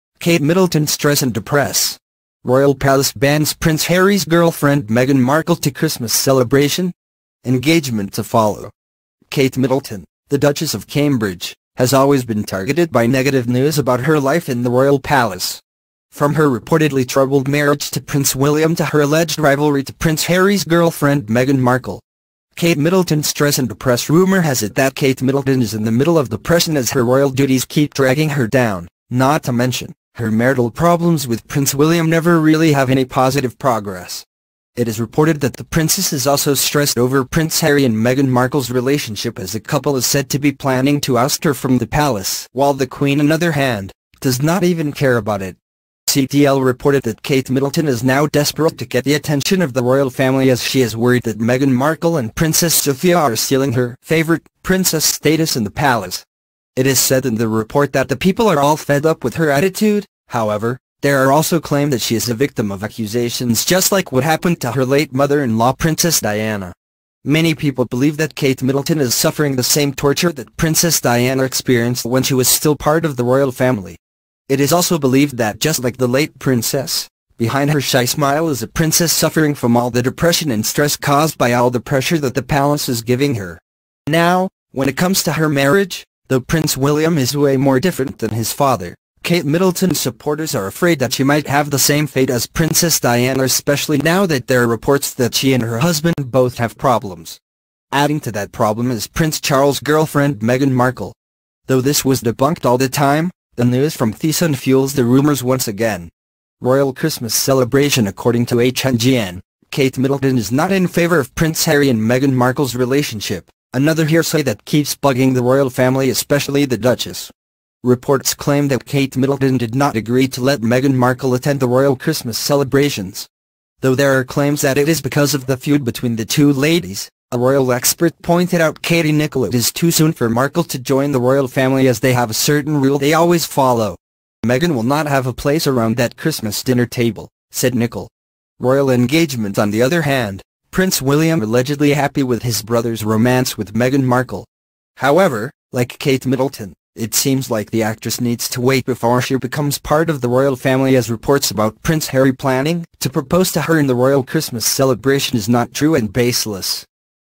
Kate Middleton Stress and Depress; Royal Palace Bans Prince Harry's Girlfriend Meghan Markle to Chr
Kate Middleton Stress and Depress; Royal Palace Bans Prince Harry's Girlfriend Meghan Markle to Christmas Celebration? Engagement to Follow.

Kate Middleton, the Duchess of Cambridge, has always been targeted by negative news about her life in the royal palace. From her reportedly troubled marriage to Prince William to her alleged rivalry to Prince Harry's girlfriend Meghan Markle.

Kate Middleton Stress and Depress

Rumor has it that Kate Middleton is in the middle of depression as her royal duties keep dragging her down, not to mention, her marital problems with Prince William never really have any positive progress. It is reported that the princess is also stressed over Prince Harry and Meghan Markle's relationship as the couple is said to be planning to oust her from the palace while the queen another hand, does not even care about it.

CDL reported that Kate Middleton is now desperate to get the attention of the royal family as she is worried that Meghan Markle and Princess Sofia are stealing her "favorite" princess status in the palace. It is said in the report that the people are all fed up with her attitude, however, there are also claim that she is a victim of accusations just like what happened to her late mother-in-law Princess Diana.

Many people believe that Kate Middleton is suffering the same torture that Princess Diana experienced when she was still part of the royal family. It is also believed that just like the late princess, behind her shy smile is a princess suffering from all the depression and stress cause by all the pressure that the palace is giving her.

Now, when it comes to her marriage, though Prince William is way more different than his father, Kate Middleton's supporters are afraid that she might have the same fate as Princess Diana especially now that there are reports that she and her husband both have problems. Adding to that problem is Prince Charles' girlfriend Meghan Markle. Though this was debunked all the time, the news from TheSun fuels the rumors once again.

Royal Christmas Celebration

According to HNGN, Kate Middleton is not in favor of Prince Harry and Meghan Markle's relationship, another hearsay that keeps bugging the royal family especially the Duchess.

Reports claim that Kate Middleton did not agree to let Meghan Markle attend the Royal Christmas Celebrations. Though there are claims that it is because of the feud between the two ladies, a royal expert pointed out Katie Nicholl it is too soon for Markle to join the royal family as they have a certain rule they always follow.

"Meghan will not have a place around that Christmas dinner table," said Nicholl.

Royal Engagement

On the other hand, Prince William allegedly happy with his brother's romance with Meghan Markle. However, like Kate Middleton, it seems like the actress needs to wait before she becomes part of the royal family as reports about Prince Harry planning to propose to her in the Royal Christmas Celebration is not true and baseless.